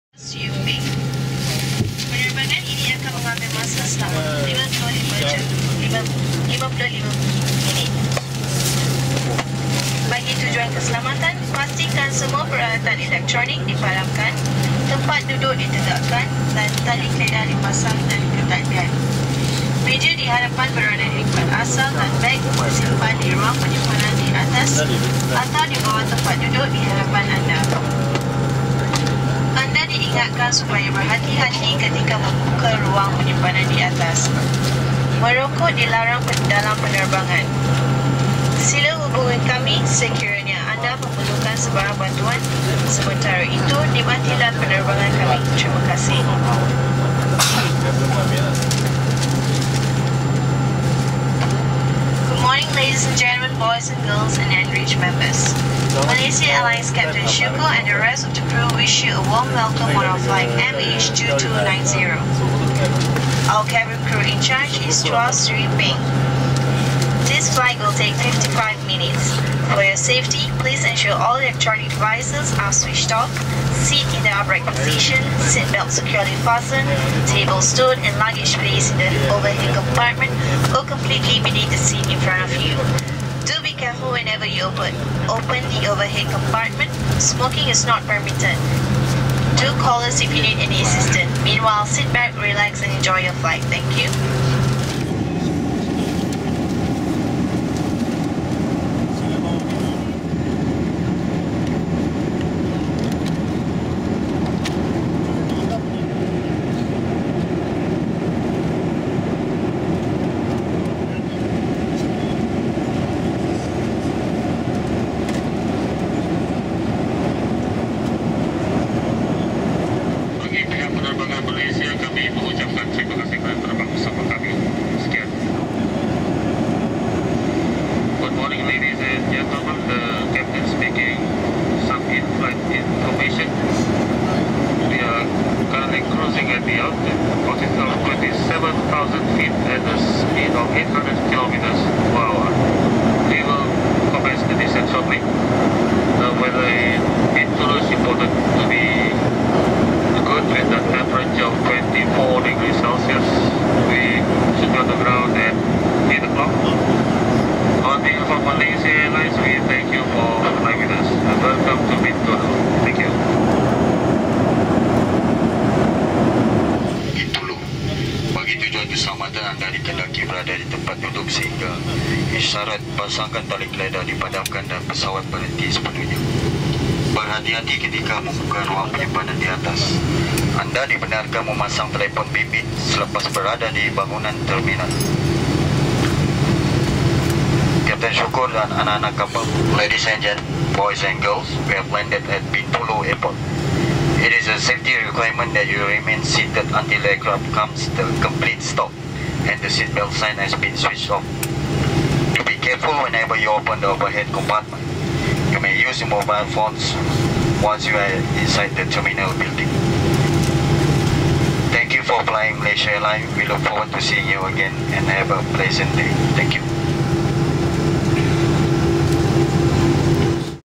Malaysia Airlines MH2290: Kuching to Bintulu, Malaysia
It was a short flight of less than one hour. I wasn't able to film much as was sitting next to a very fearful individual.